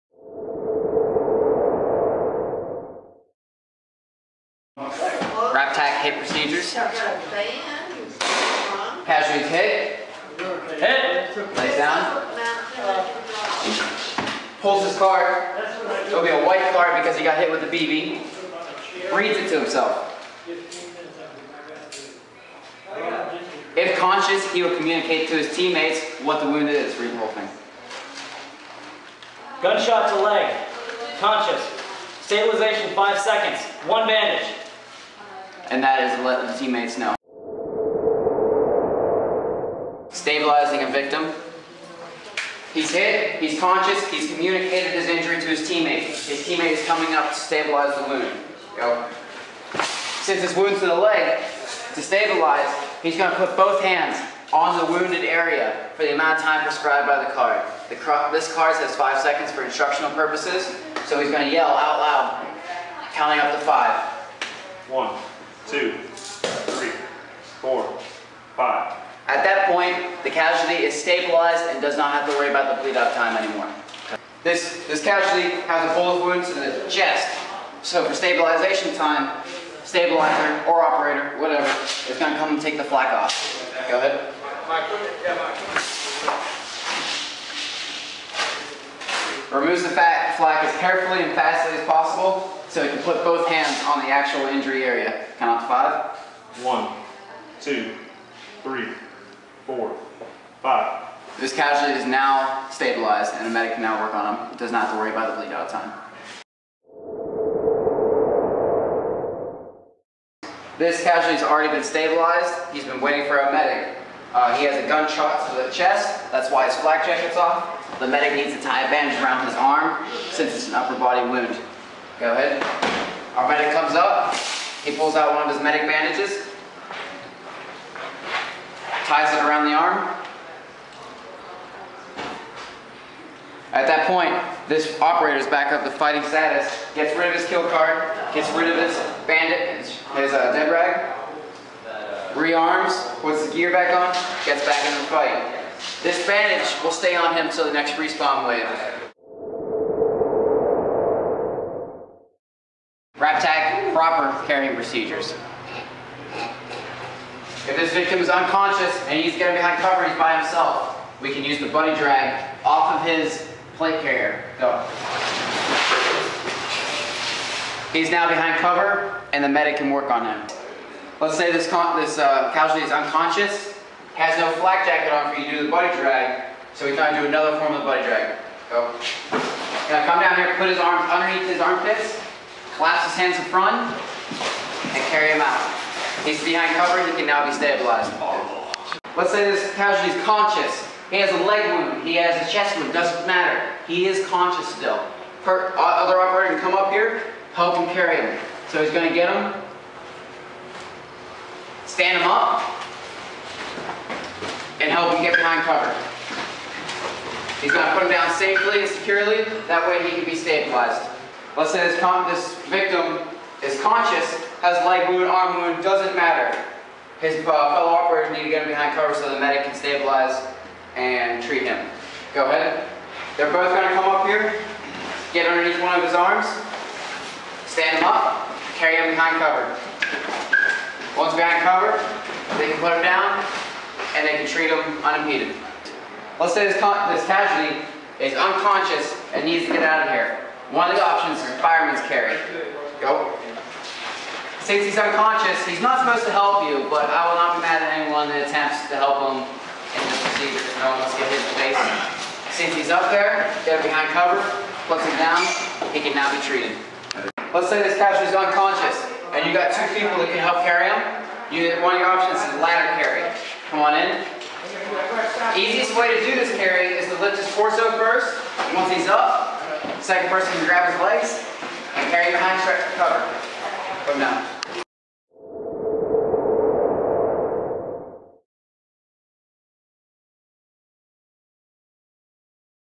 RAPTAC Casualty Procedures
Casualty procedures for all Raptor Tactical Applications and Operations (RAPTAC) events. This video is intended to be a brief instructional video and to answer many of the frequently asked questions. It is recommended to view this video at least once prior to attending any of our events.